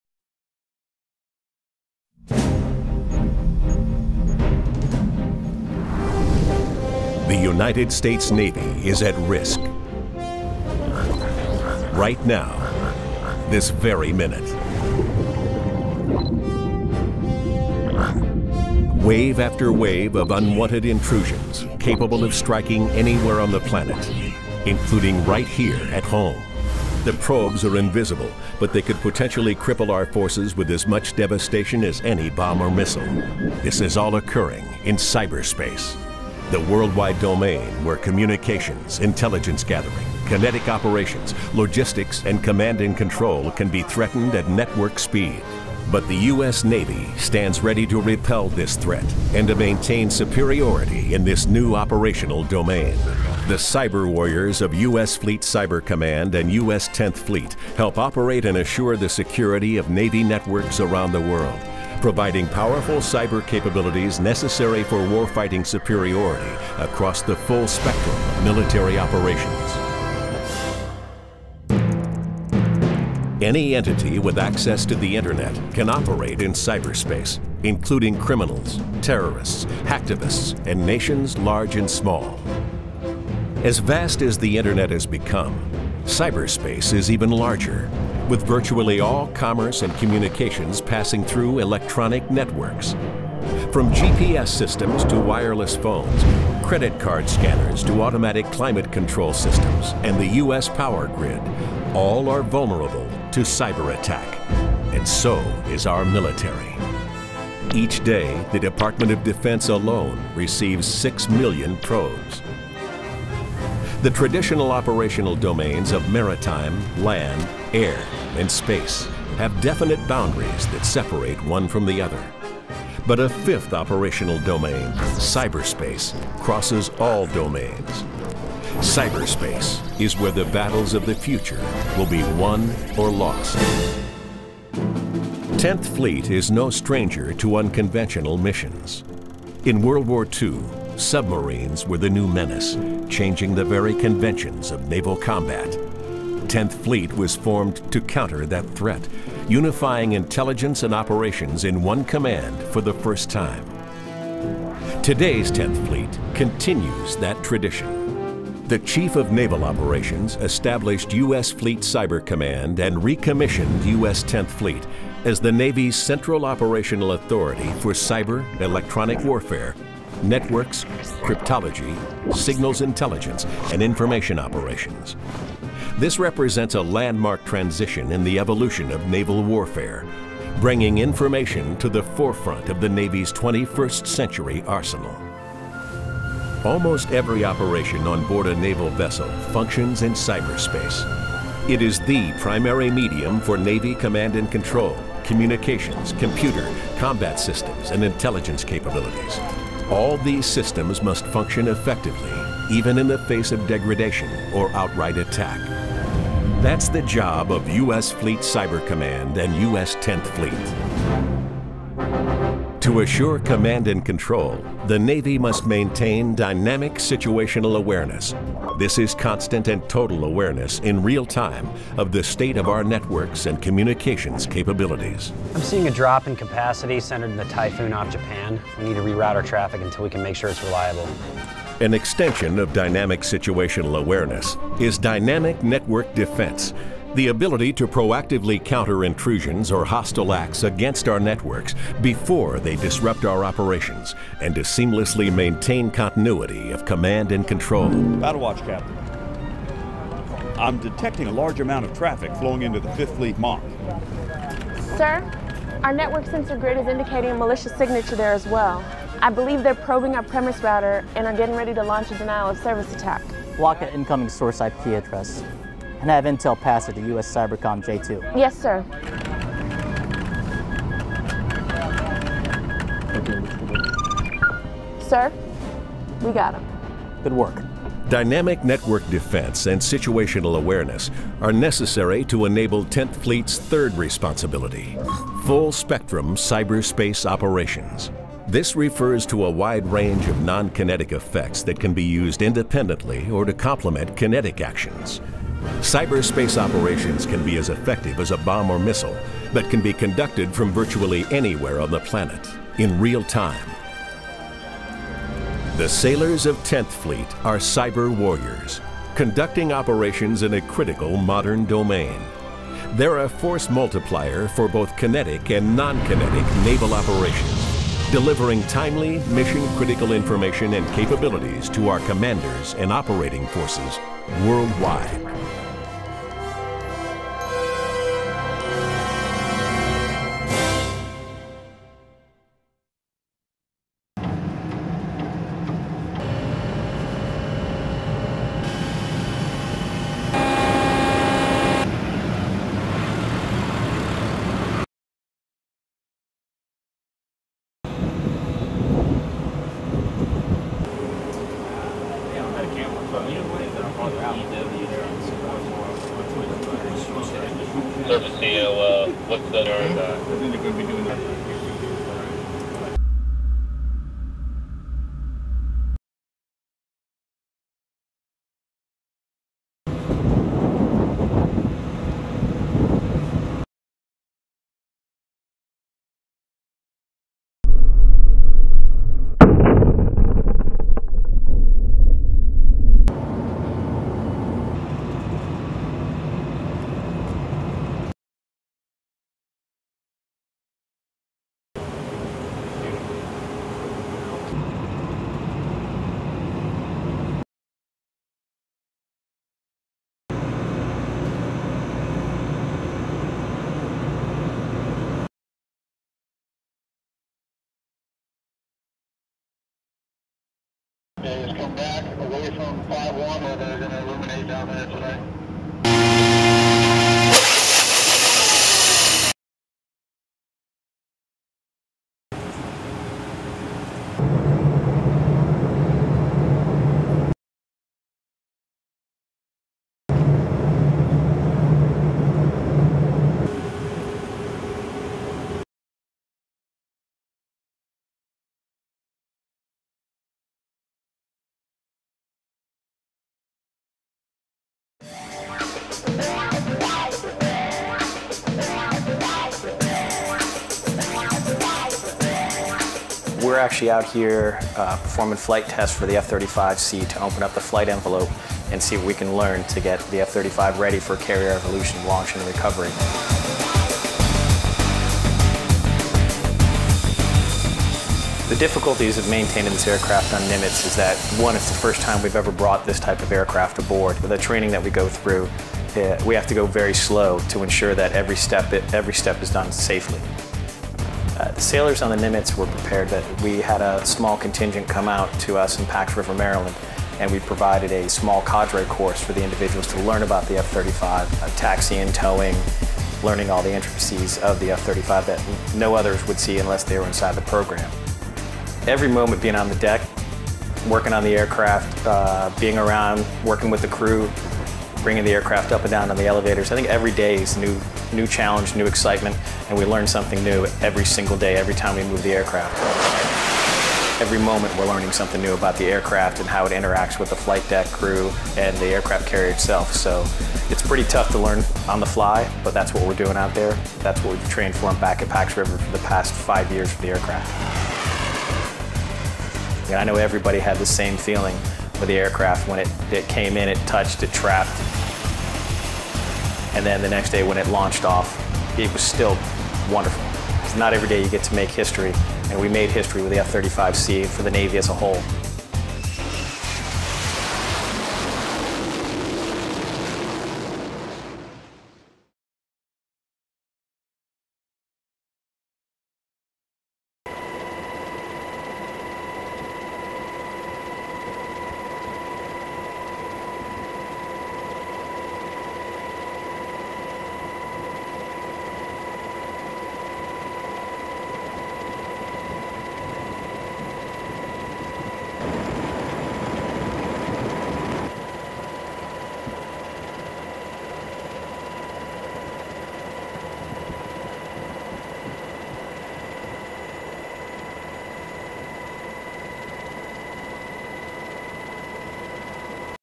Future US Navy Technology - Cyber Warfare - Laser Weapons - Advanced Carrier Fighter Jets
United States Navy Military Technology Preview
Cyber Warfare Command And Control Systems Electronic Warfare
USS Ponce Laser Weapon System Test Against Ship And Air Targets
F35 Carrier Test Flights Landing Advanced Navy Fighter Jet Technology
Old Tall Wooden Ship Escorted By Modern Navy Destroyer